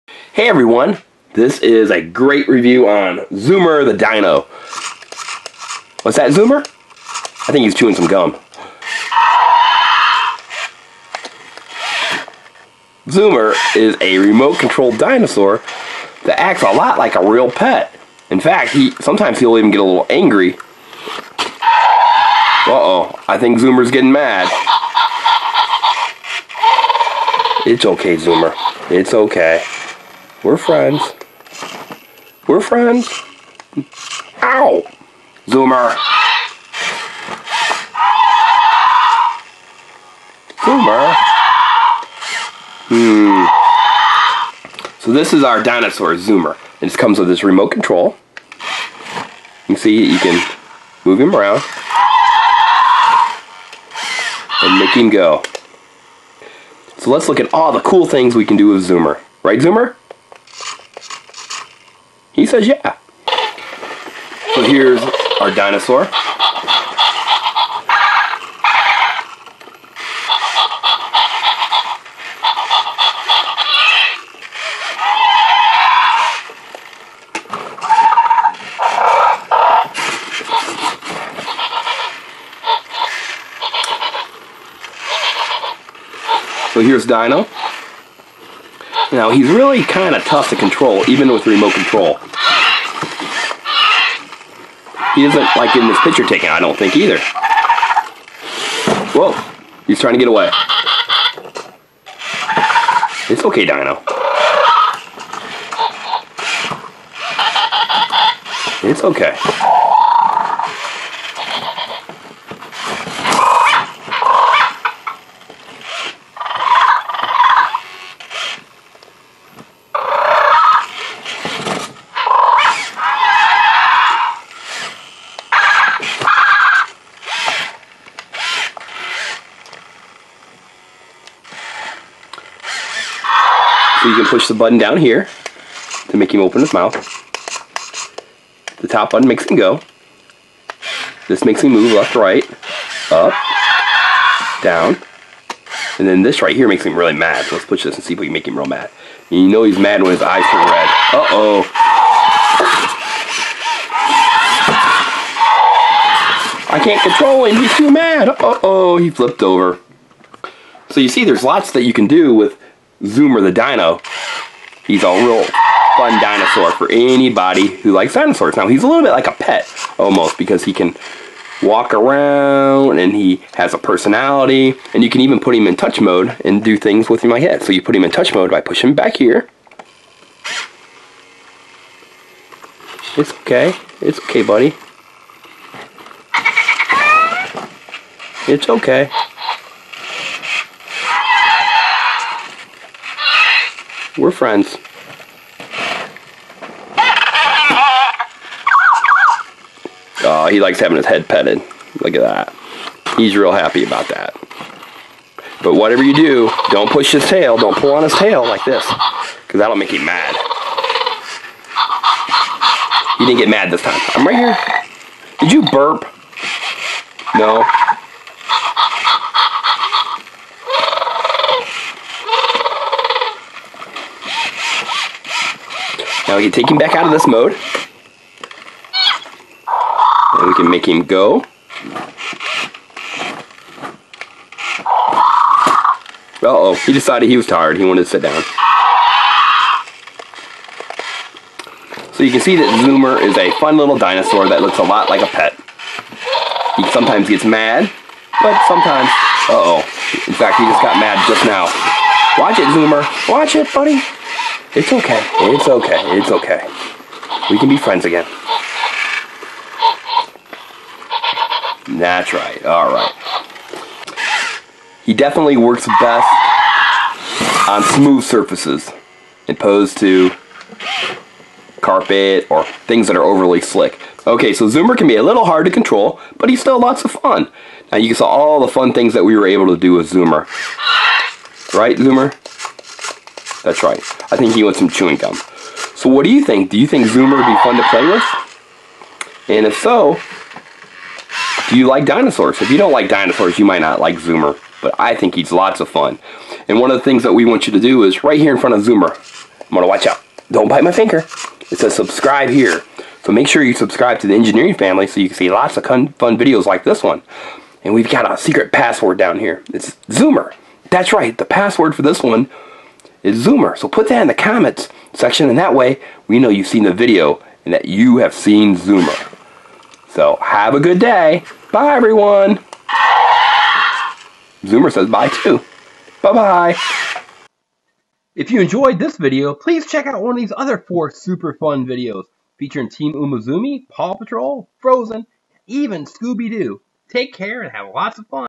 Unboxing Zoomer The Robotic Pet Dinosaur by TheEngineeringFamily
Zoomer The Robotic Pet Dinosaur is an awesome toy!

Spinmanster has created Zoomer the robotic dinosaur. Zoomer looks and acts like a real pet dinosaur - he can be sweet like a puppy, but he can also get upset (watch when his eyes turn red). In this video we see Zoomer and how we can interact with him. Now Zoomer can be a bit difficult to control but he is still lots of fun.

Please Note I received this product for reviewing purposes

In this Playlist The Assistant goes on adventure with Scooby Doo, Paw Patrol, Funny Pig, Disney Frozen, and Doc McStuffins